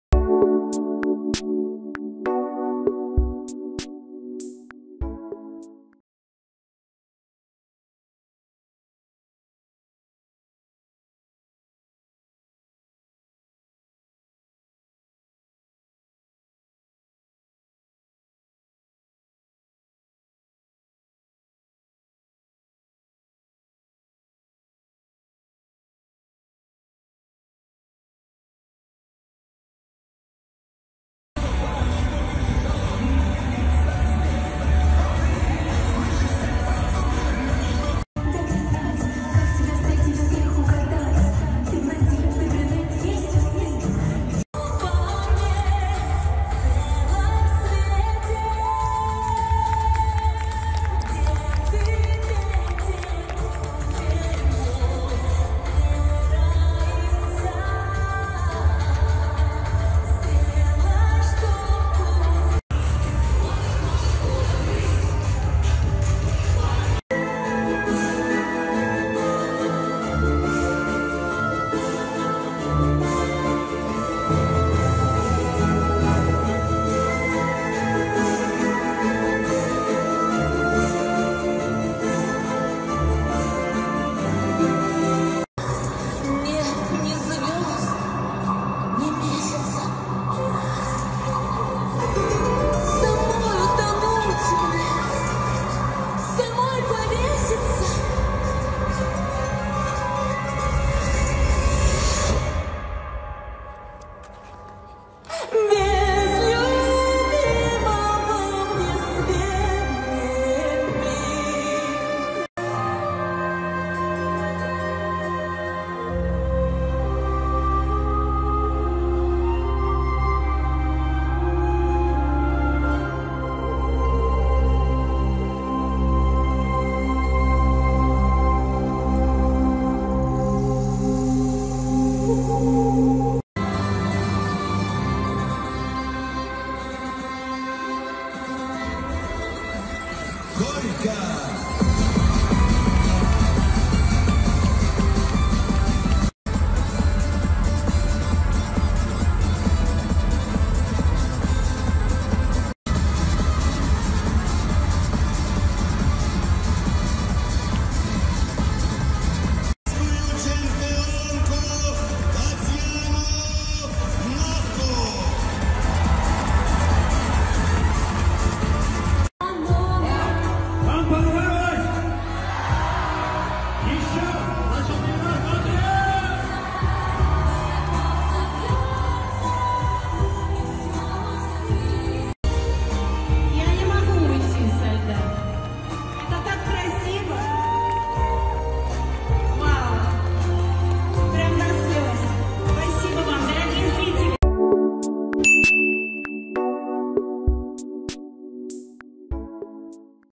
Татьяна Навка представила фееричное ледовое шоу Вечера на хуторе
В Москве на арене «Мегаспорт» в новогодние праздники проходит ледовое шоу Татьяны Навки «Вечера на хуторе» по мотивам повестей Николая Гоголя. В спектакле участвуют одни из самых ярких фигуристов: чемпионы мира в танцах на льду Виктория Синицина и Никита Кацалапов, четырехкратный чемпион России Максим Ковтун и чемпионка Европы Камила Валиева.